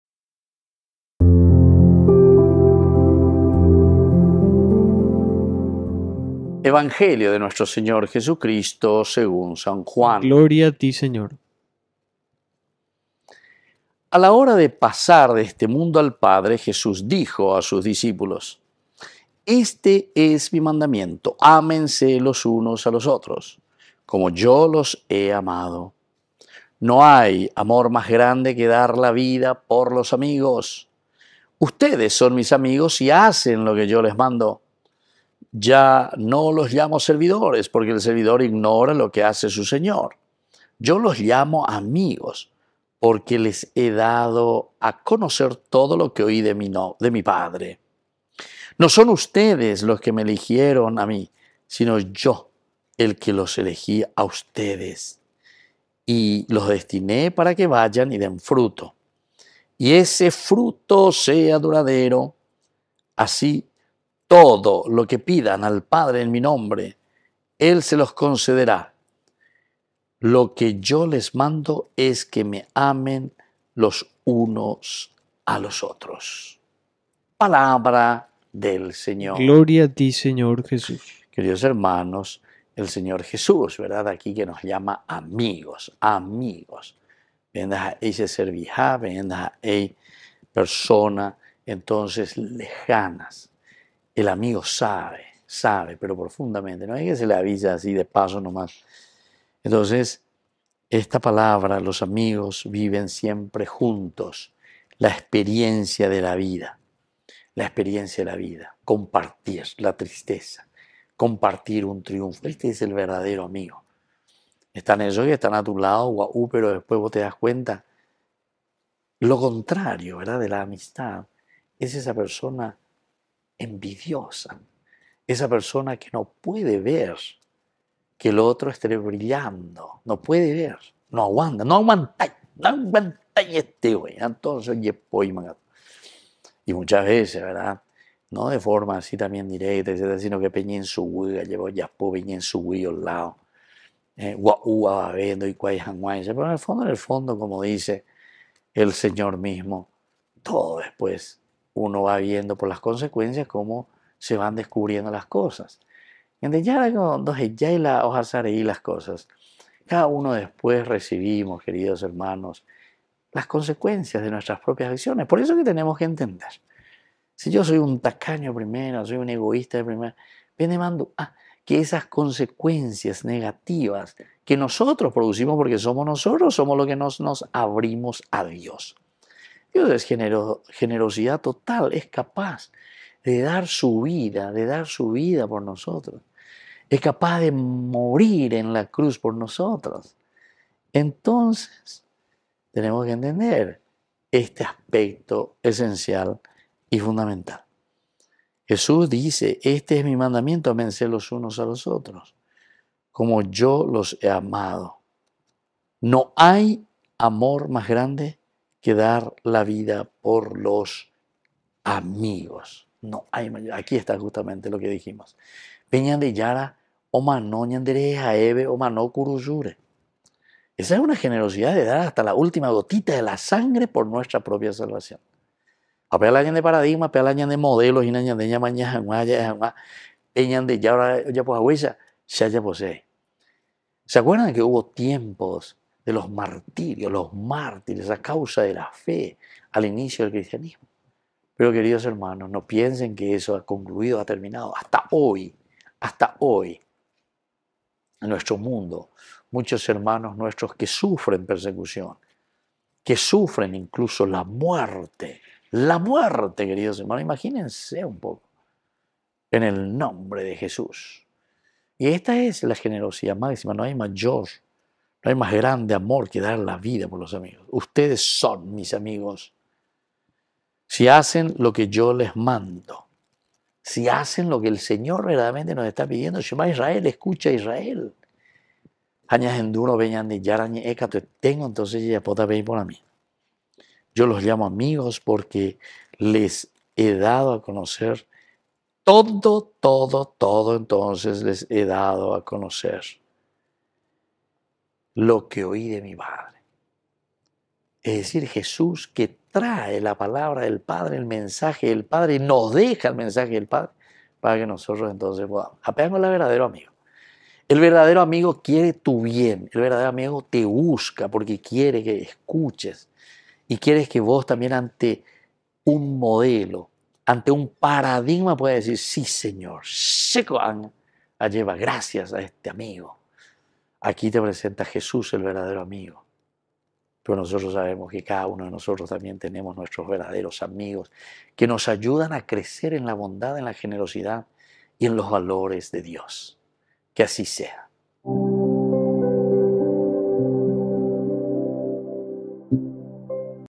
HOMILIA VIERNES 12 DE MAYO DEL 2023
Santo Evangelio según San Juan 15, 12-17: “Jesús dice que debemos amarnos el uno al otro “así como Él nos ha amado”, dando su vida por nosotros en la cruz.”